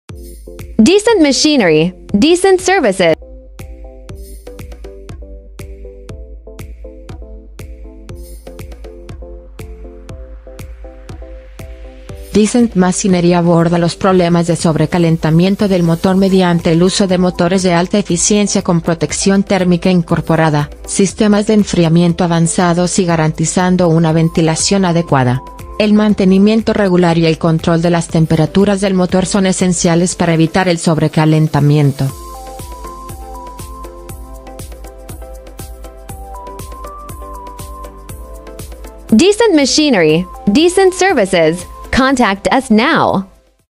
¿Cómo evita DECENT MACHINERY el sobrecalentamiento del motor en sus ventiladores?
En este video, te mostramos cómo DECENT MACHINERY aborda el problema del sobrecalentamiento del motor en sus ventiladores. Descubre las soluciones y tecnologías implementadas para asegurar un rendimiento óptimo y prolongar la vida útil de tus equipos.

Para más información, visita nuestro sitio web: Decent Machinery.

Conoce más sobre nuestra fábrica viendo el recorrido completo aquí: Recorrido por la fábrica